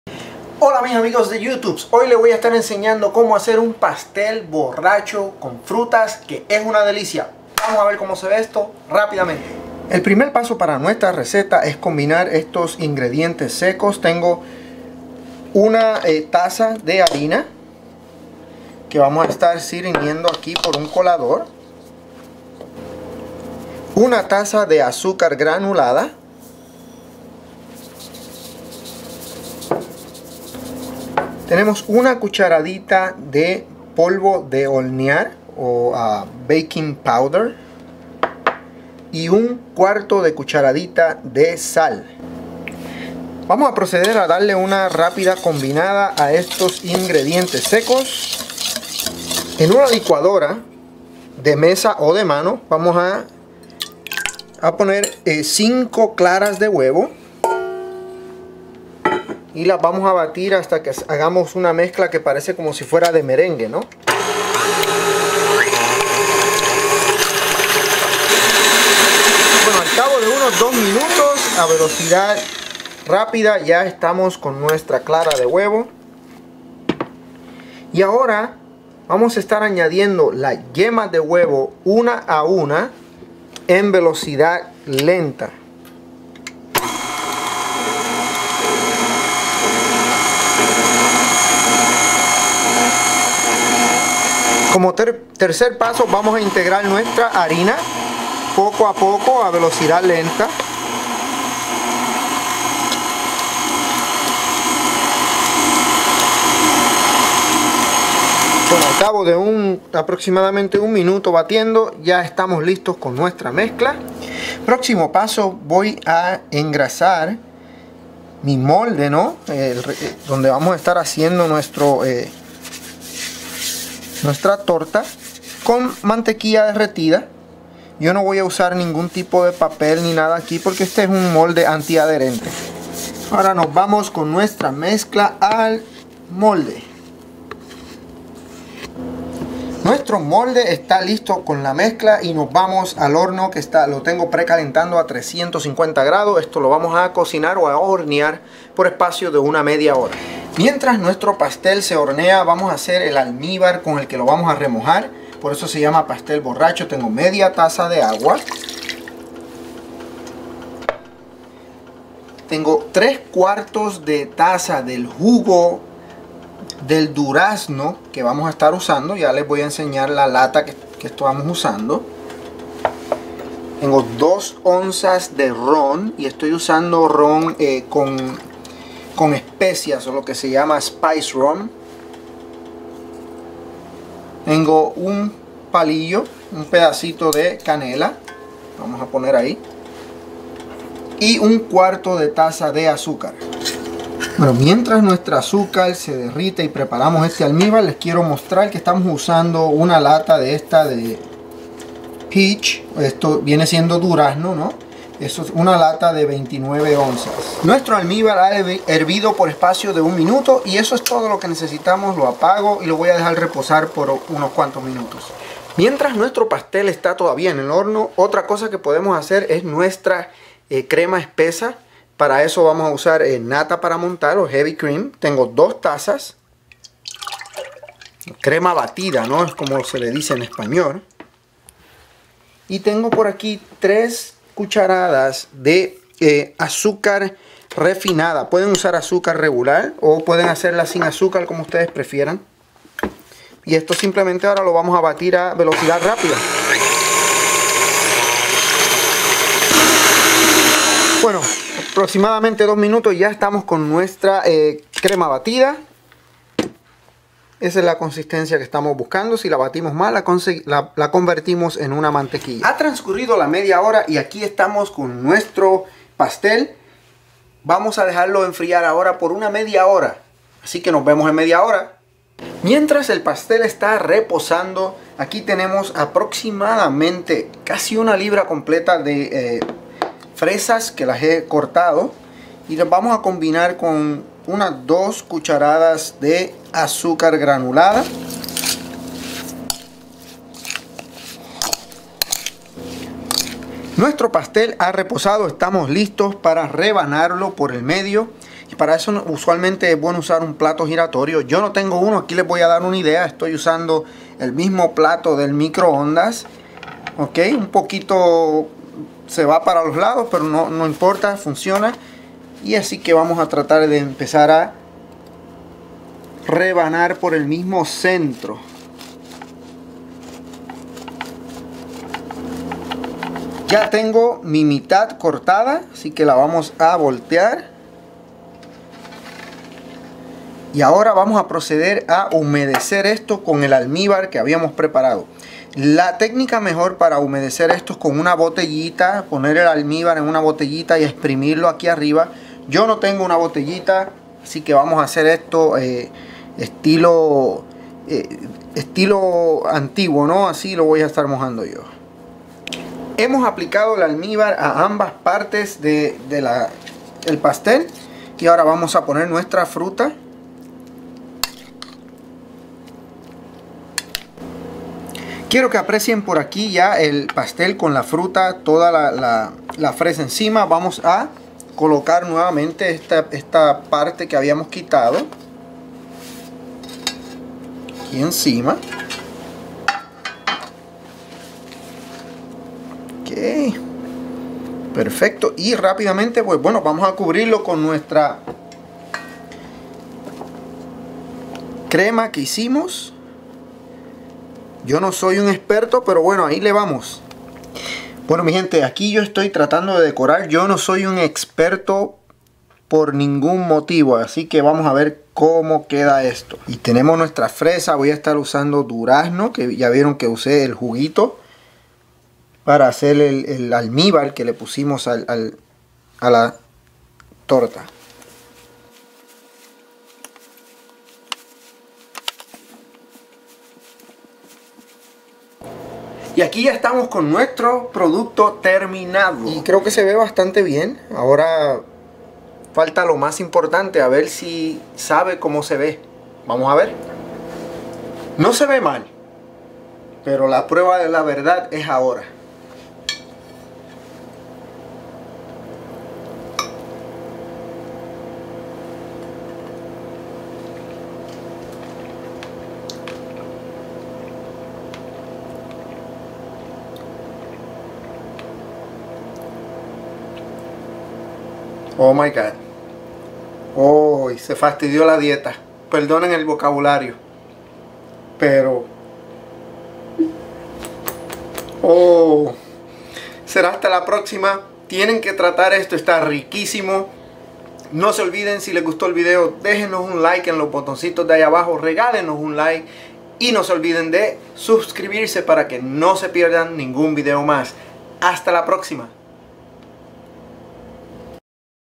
Receta Para Hacer Pastel Borracho Con Fresas Y Durazno (Melocotón)
Receta de como hacer o preparar pastel, torta o bizcocho borracho con frutas y crema batida.